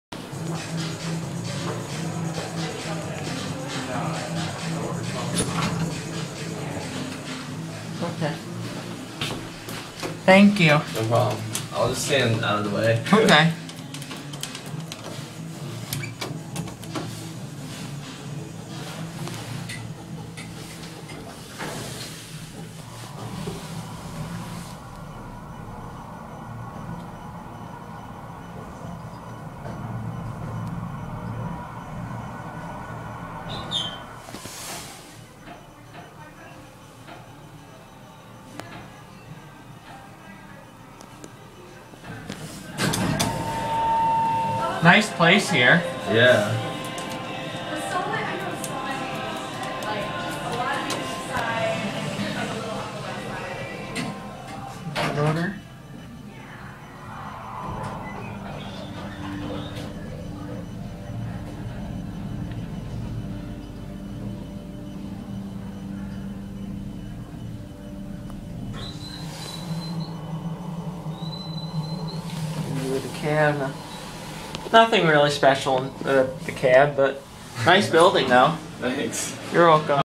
?? Hydraulic Elevator At An ?? Location In NYC
This is the ?? Hydraulic elevator at An ?? Location in NYC. Thank you to the gentleman who let me ride and video this.